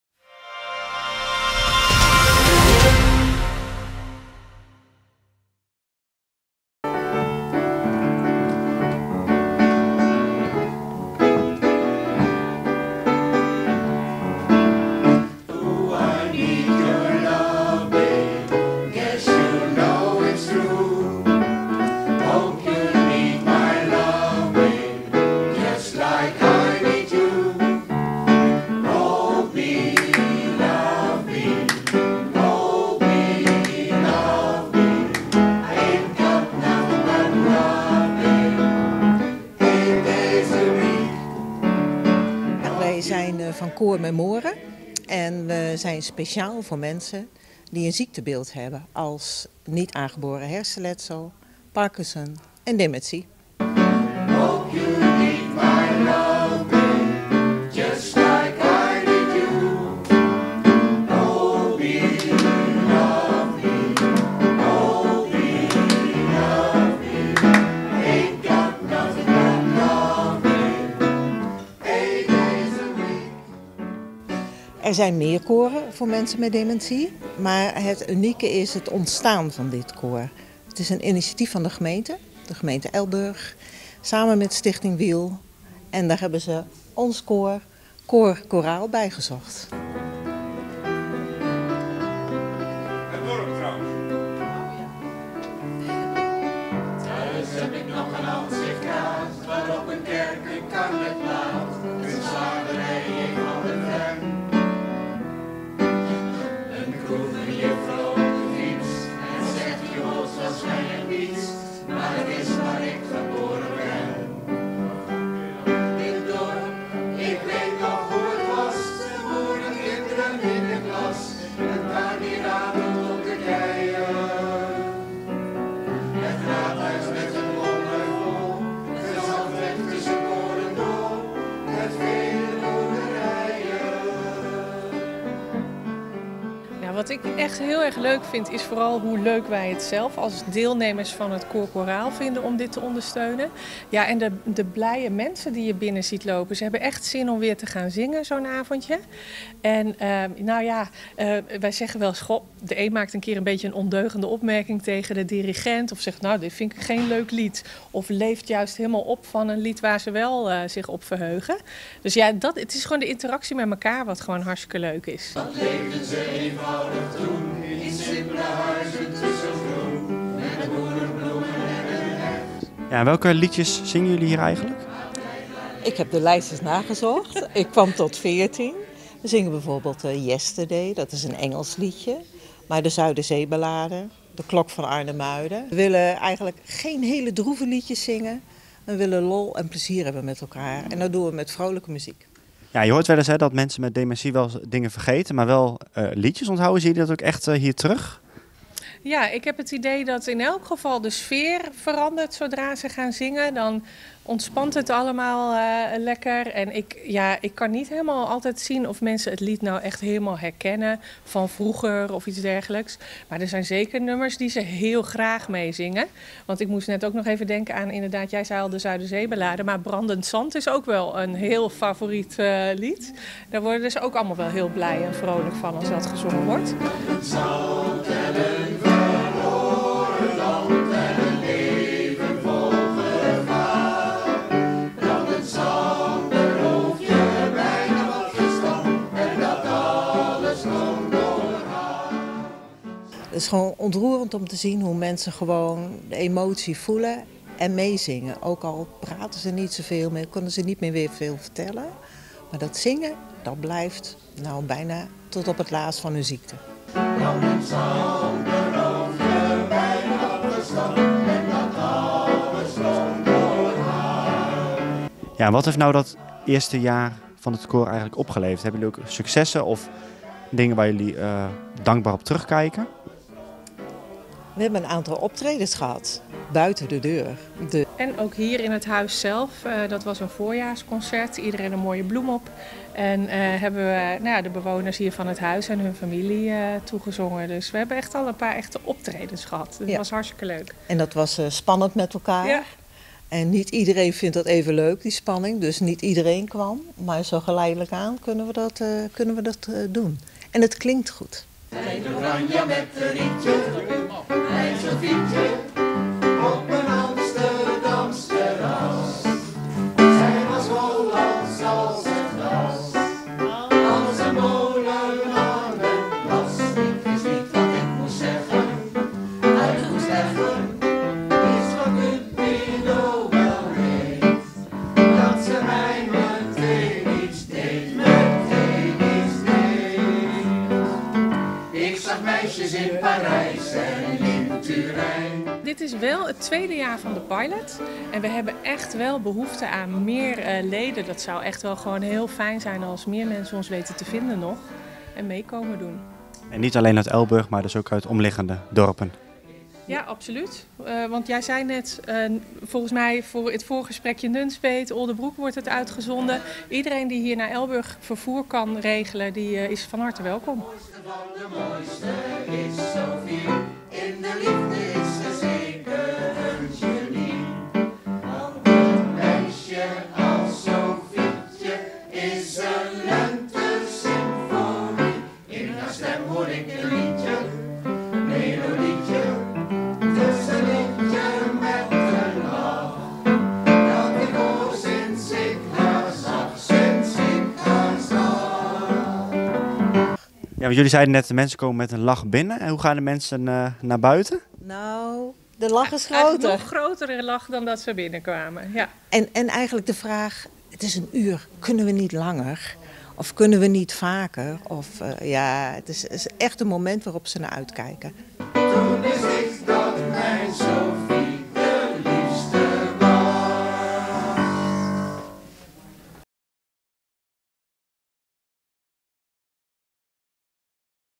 Memore, koor voor mensen met hersenbeschadiging, zingt in De Voord
Myrthe van Eijsden, Eljakim Doornewaard en Henk Tabois